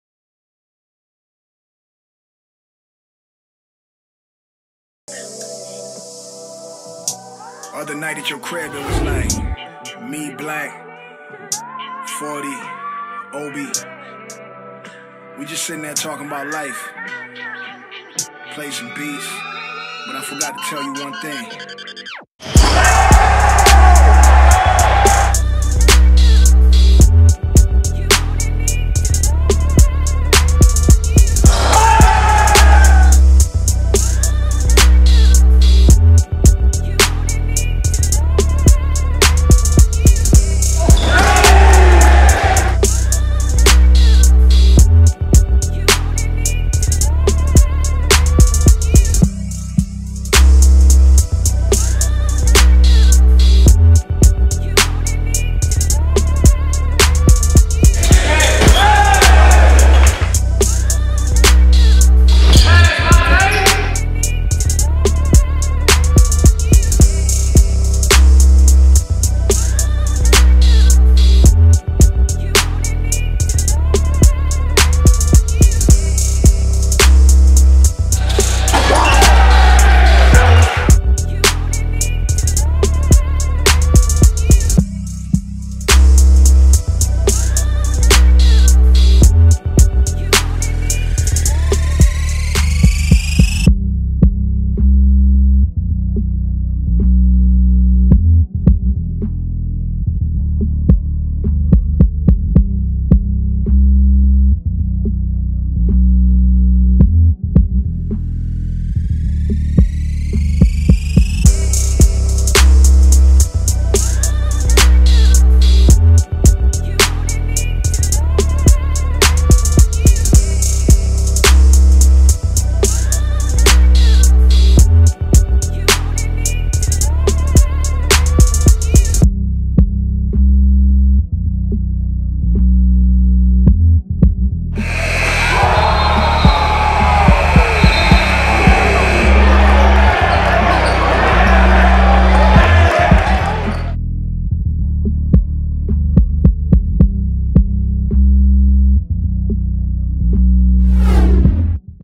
Team Thad Keeps It 100 In Vegas
Highlights of the best moments from Team Thad's FAB 48 appearance.